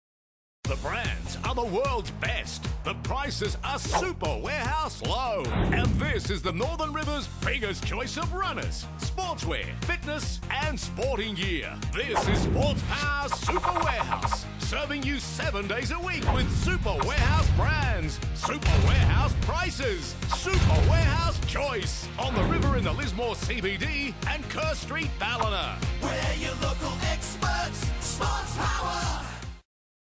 Super Warehouse Prices at SportsPower Super Warehouse
Sportspower Super Warehouse brings you Super Warehouse prices on Nike, Asics, and Brooks running shoes, Lorna Jane, Running Bare and Champion apparel, and York Fitness and BodyWorx fitness equipment. Open 7 days a week in Lismore CBD and Kerr Street Ballina.